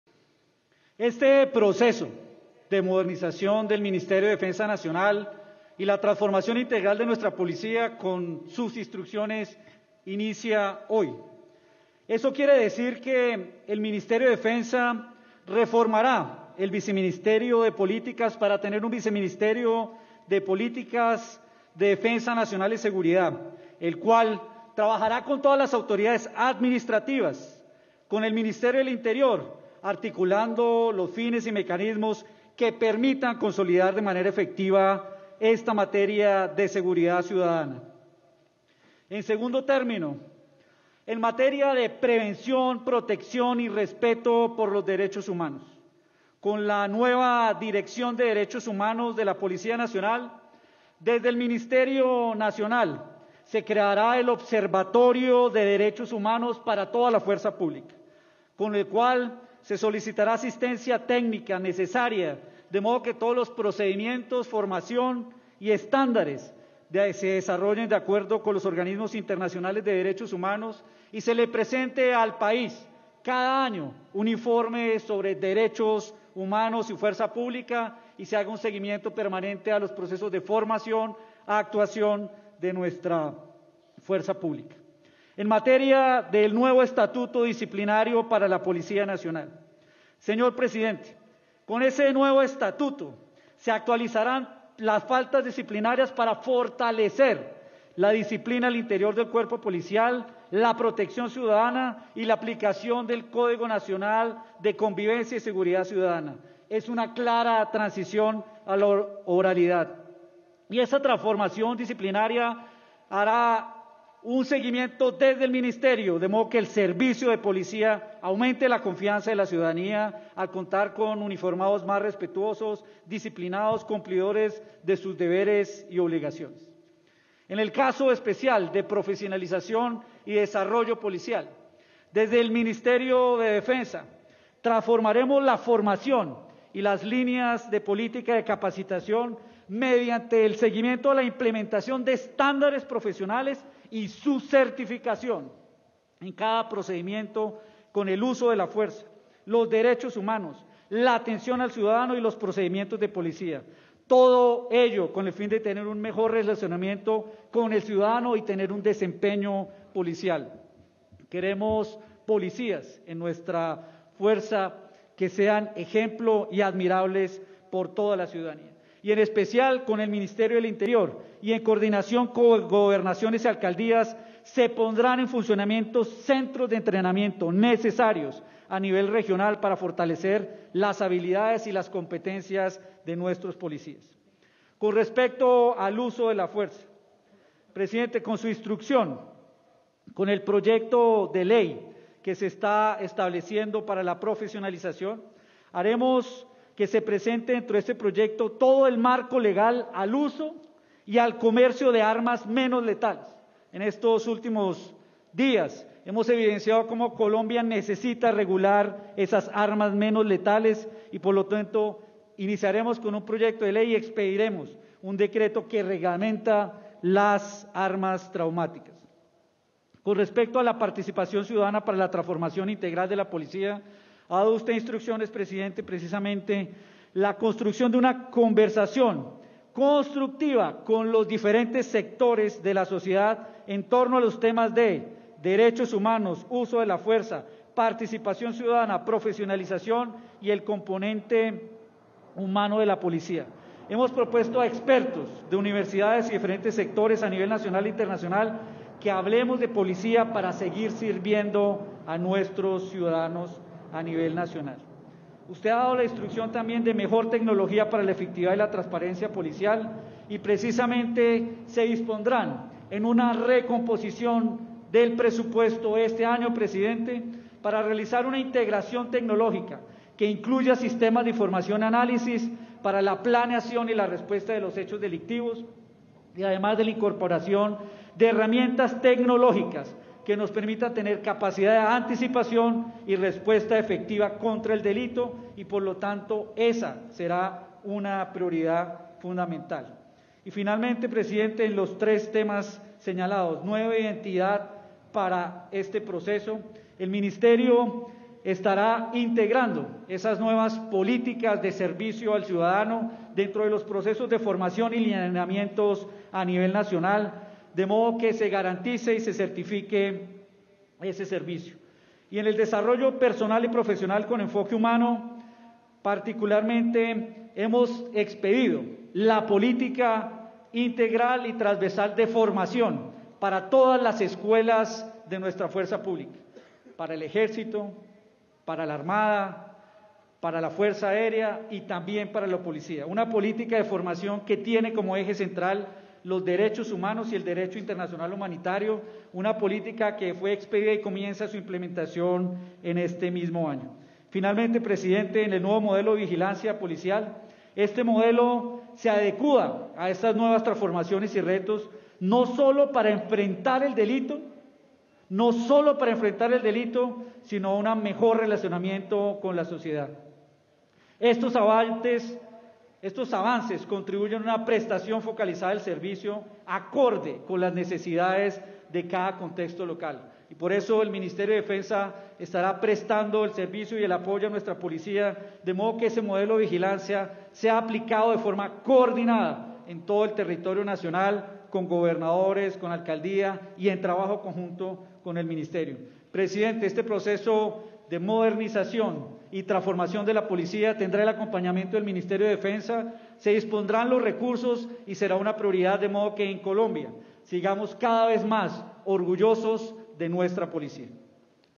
Reforma integral de la Policía Nacional
El ministro de Defensa, Diego Molano, entregó detalles de la modernización en la estructura del Ministerio de Defensa y la reforma a la Policía Nacional.